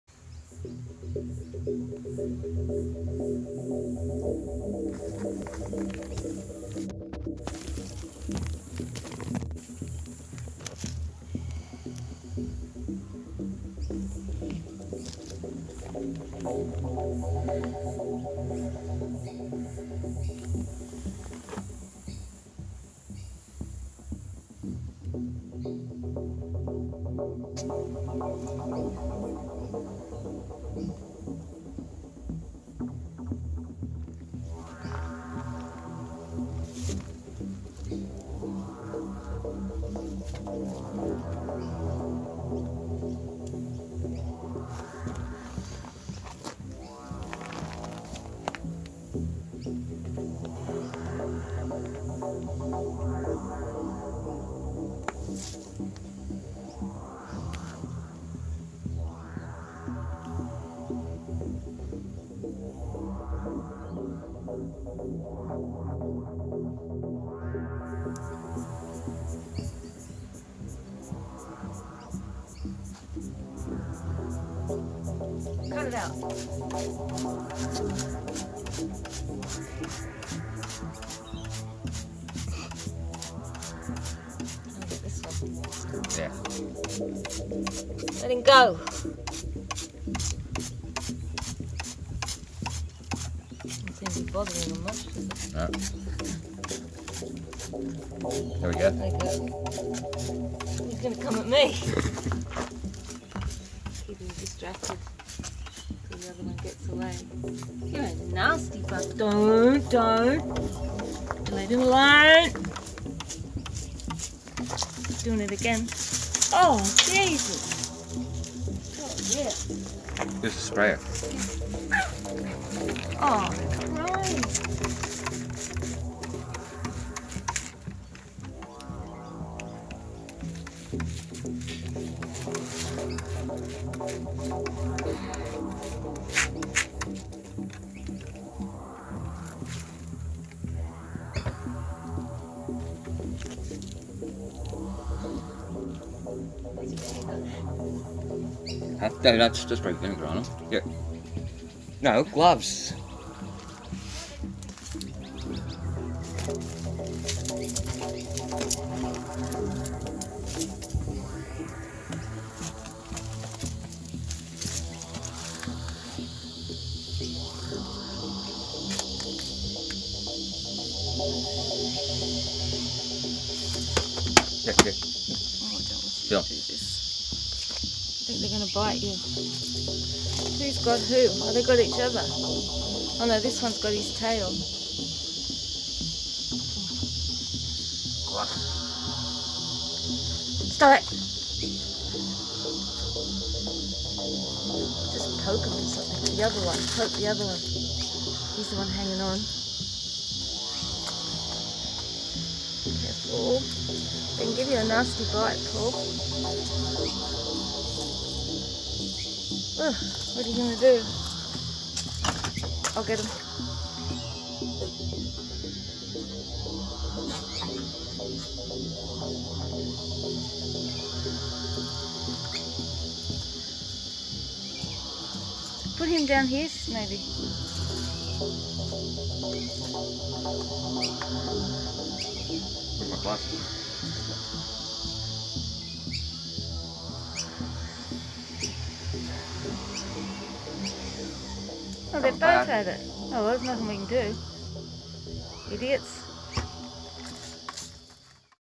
Battle of The Blue Tongues 20210115
Found a couple of Blue Tongue lizards having a battle at the side of my house.
Tried to separate them but they were very intent in establishing their territory, after the vid put sent them to their respective naughty corners, one in the backyard & one in the front yard.